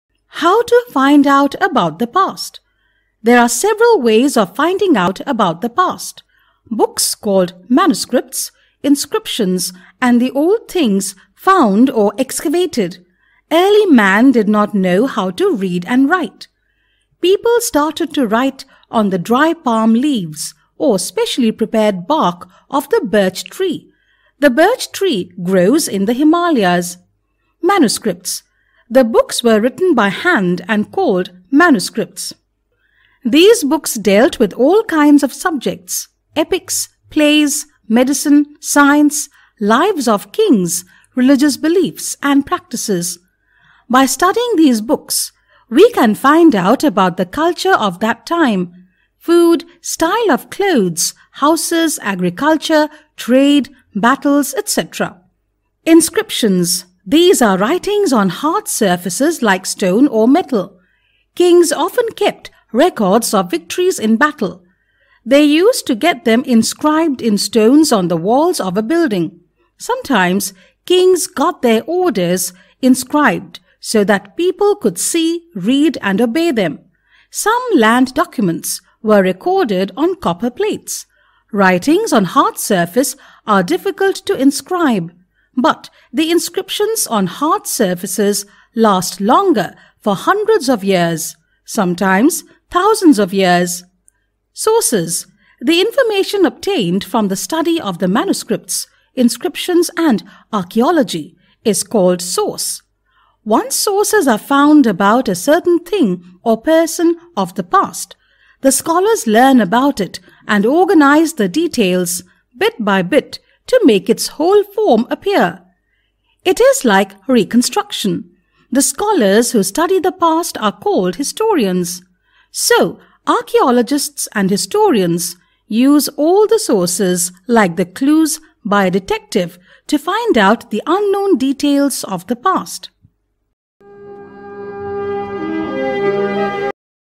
Finding Out About the Past | What, Where, When and How? | Class 6 History
How to find out about the Past- Class 6 History
In this video we will learn about - Finding Out About the Past, its various aspects and how it affects you.
Useful For: NCERT / CBSE / ICSE / Olympiad / NTSE